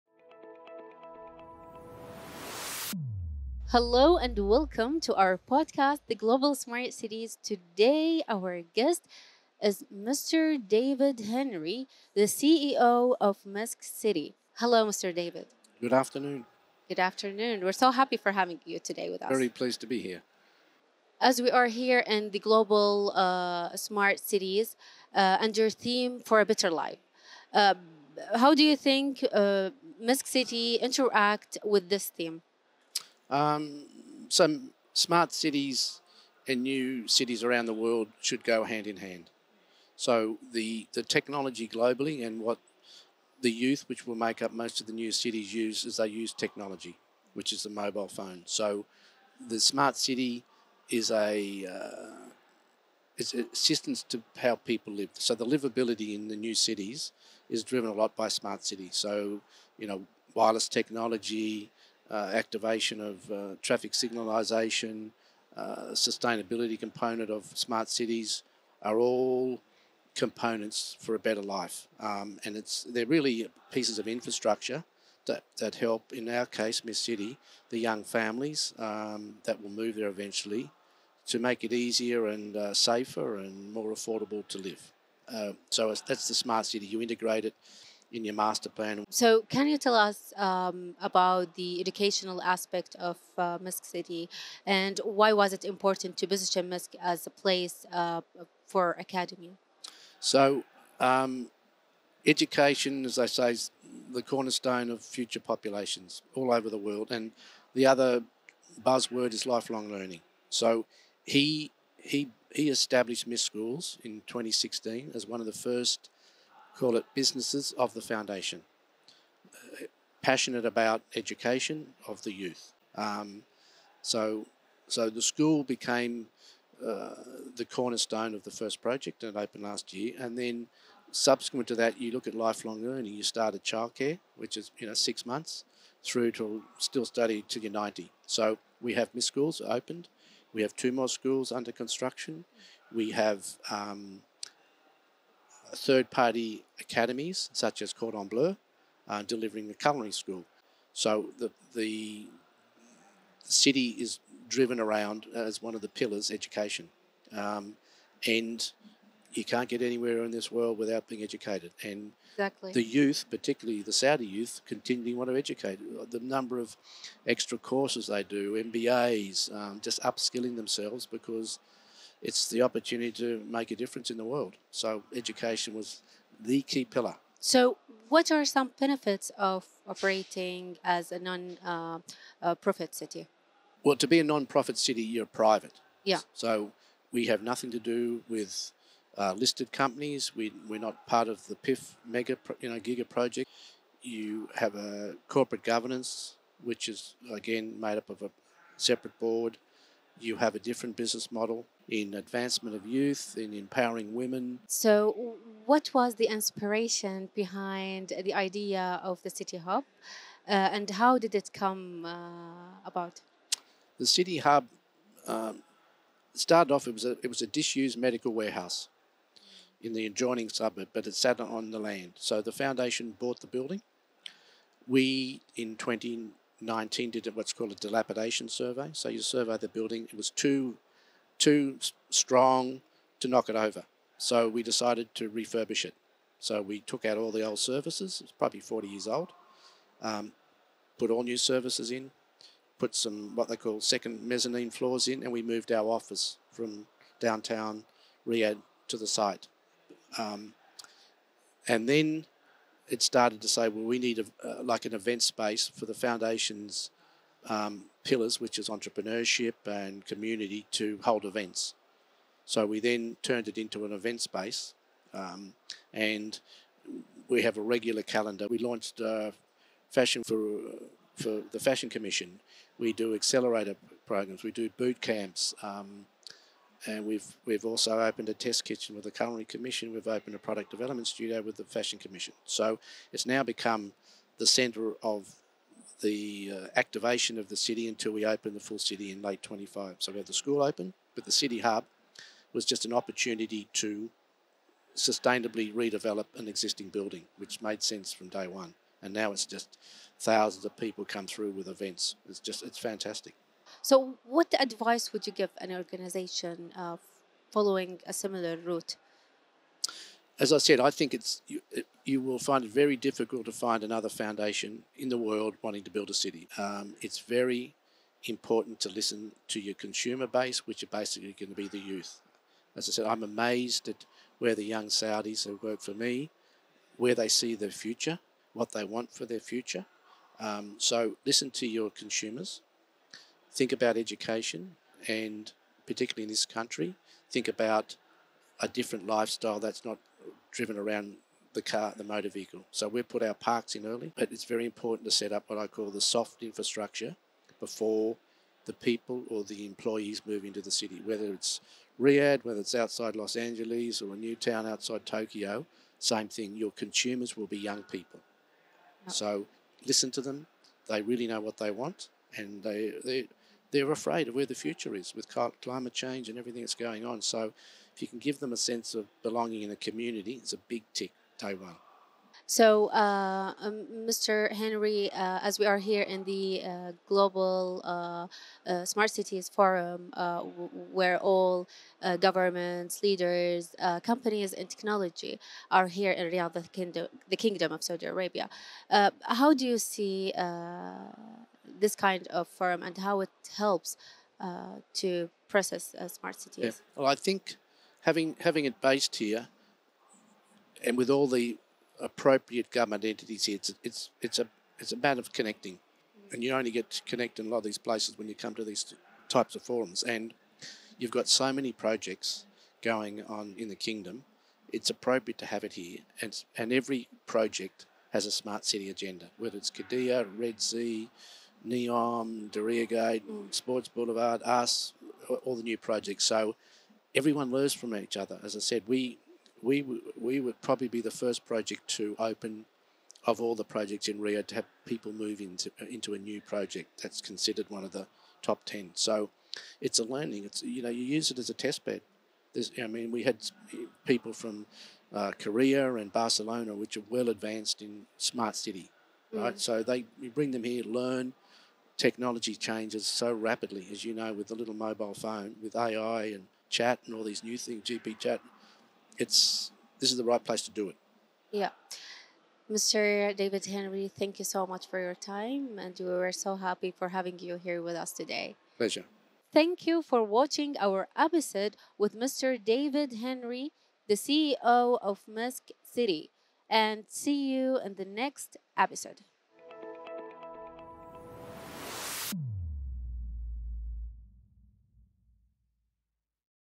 Building Better Futures: Inside the World's First Non-Profit City
In this episode, we explore the unique challenges faced in creating the first nonprofit city, the educational initiatives within Misk City, the advantages of operating as a nonprofit entity, the genesis of the innovative City Hub idea, and valuable advice.